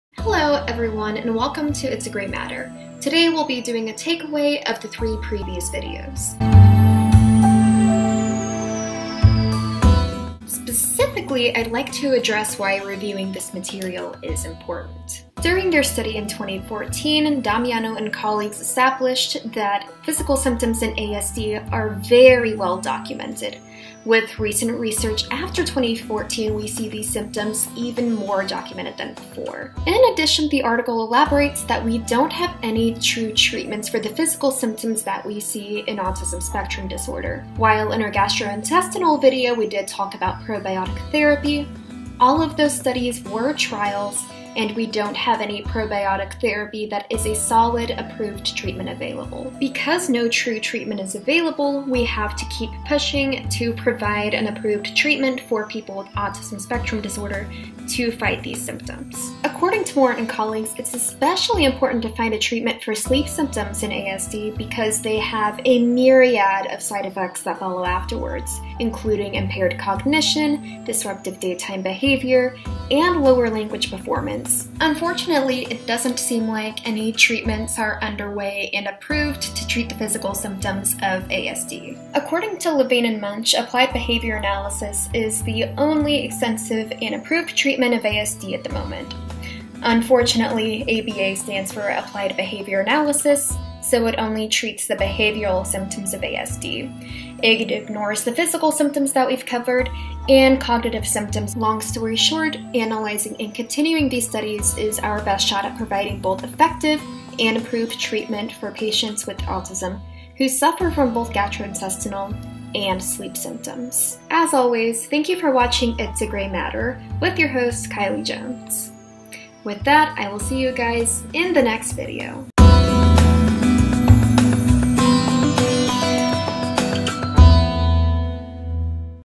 Takeaway and Application of ASD Symptom Research | It's A Gray Matter
Hello, and welcome! This video explains the importance of the previous three. As always, this content is only for educational and not commercial purposes. Full APA-style 7th-edition references are below:

Damiano, C. R., Mazefsky, C. A., White, S. W., & Dichter, G. S. (2014). Future directions for research in autism spectrum disorders. Journal of Clinical Child & Adolescent Psychology, 43(5), 828-843.
Levine, L. E., & Munsch, J. (2022). Child development: An active learning approach (4th ed.). SAGE Publications, Inc. 173.
Moore, M., Evans, V., Hanvey, G., & Johnson, C. (2017). Assessment of sleep in children with autism spectrum disorder. Children, 4(72), 1-17.